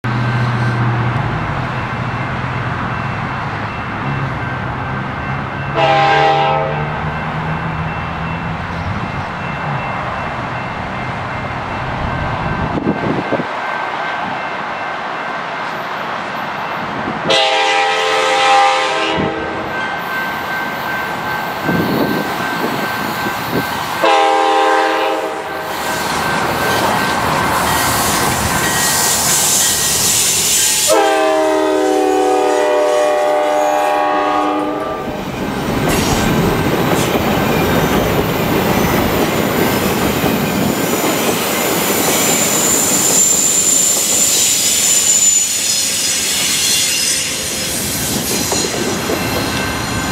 Amtrak 90250 Leads Cascades 516 at Everett, WA 8-12-13
Amtrak NPCU 90250 leads a 20-minute-late Cascades train 517 at Pacific Ave. in Everett, WA with P42DC #147 trailing. The train has already made its station stop at Everett and is heading back  towards the Bellingham Sub to continue its journey north.